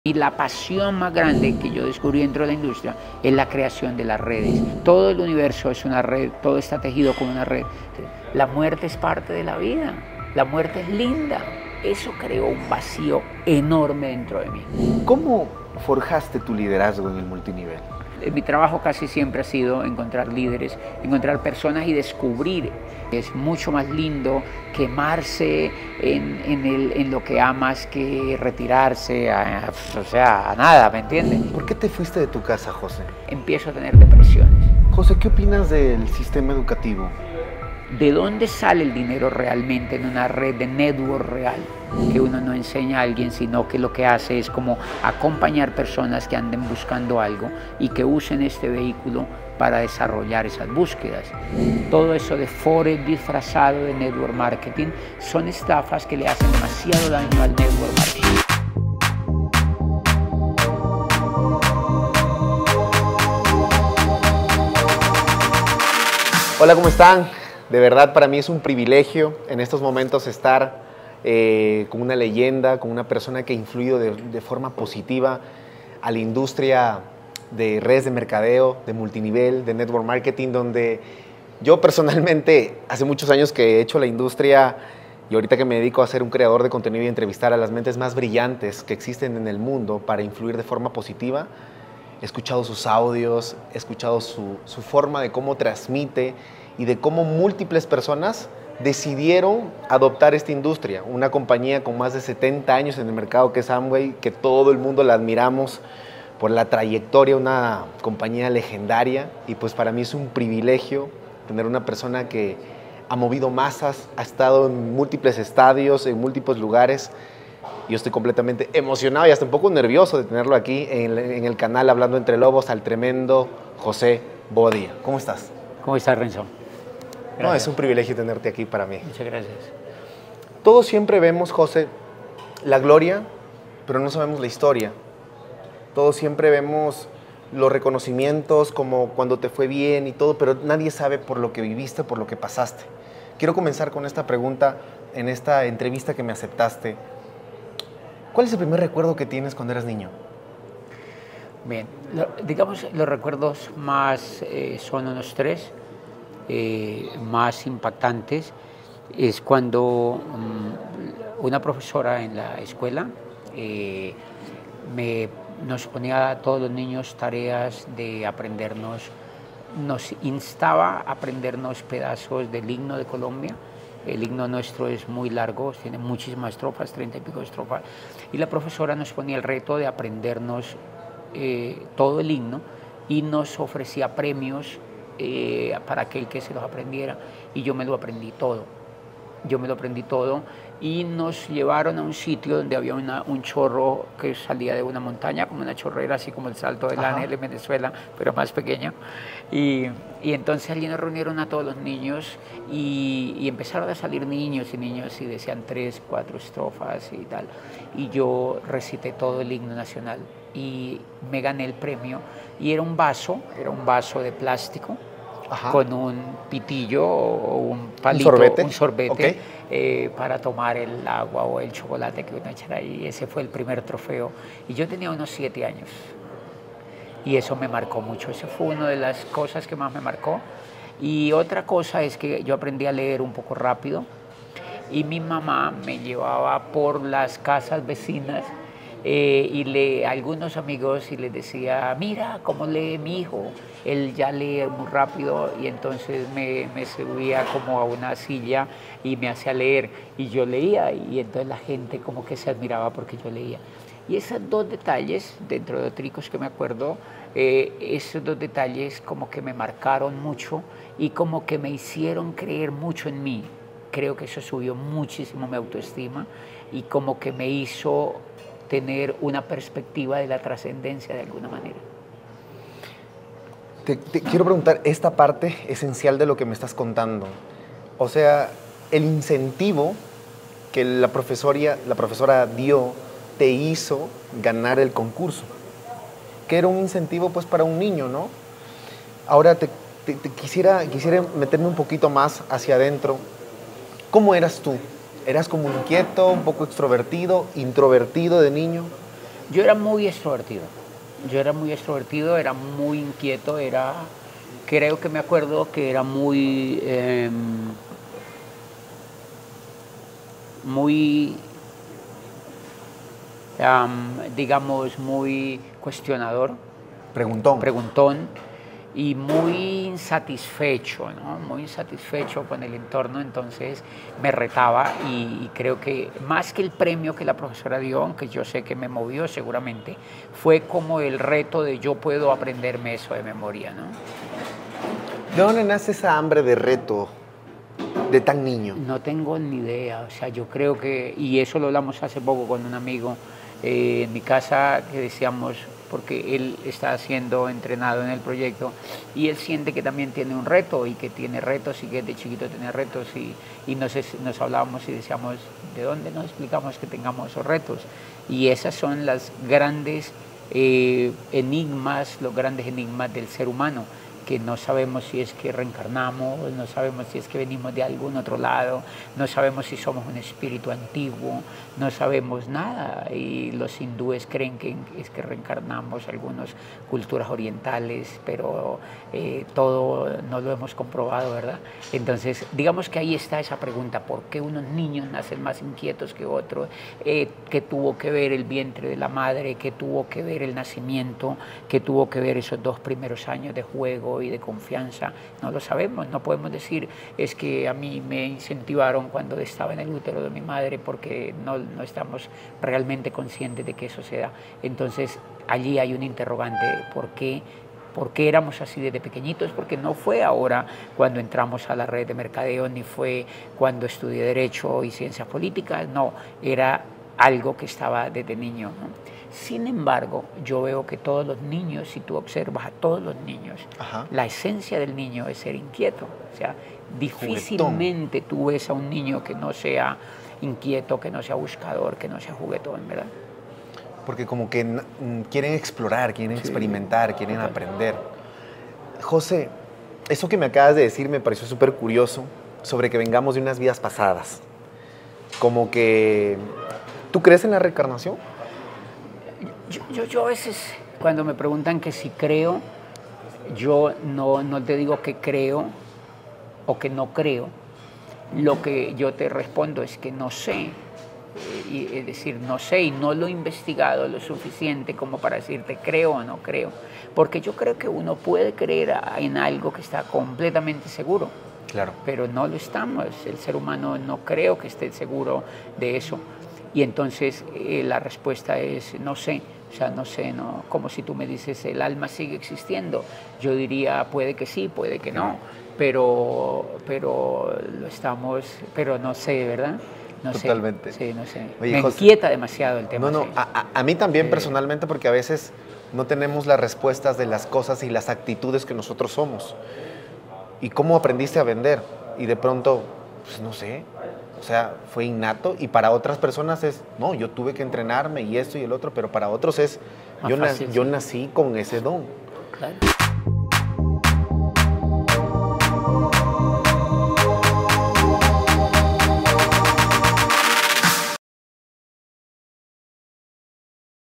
CREENCIA Y AUTOESTIMA - ENTREVISTA A JOSÉ BOBADILLA
Fragmento de la entrevista para la industria del Network Marketing

Aprendiendo de Educación, Éxito, Liderazgo